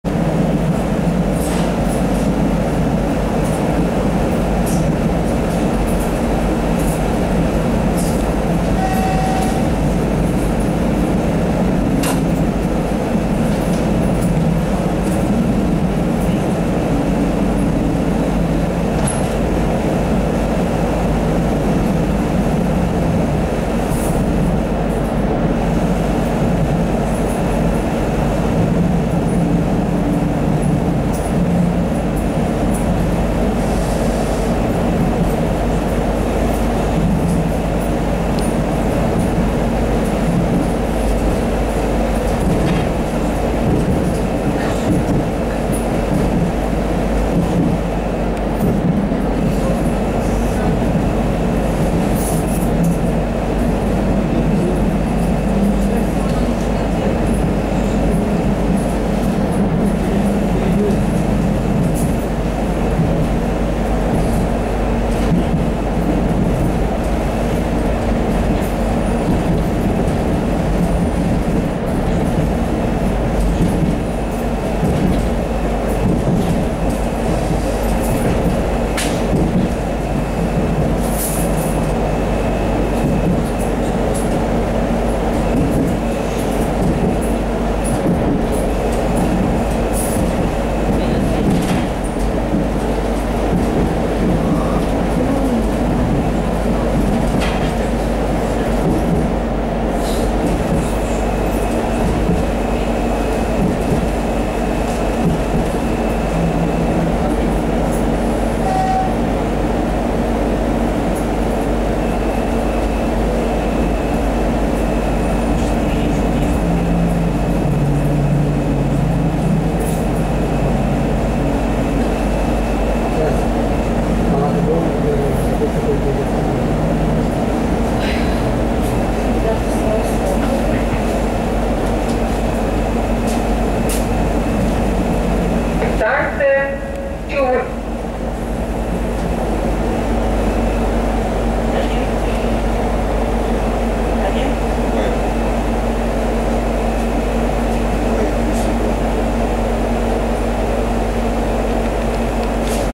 Прибытие на ст. Чур из окна рельсового автобуса
Прибытие на ст. Чур из окна рельсового автобуса РА3-032 сообщением Ижевск - Глазов
Россия/Удмуртия
12.07.20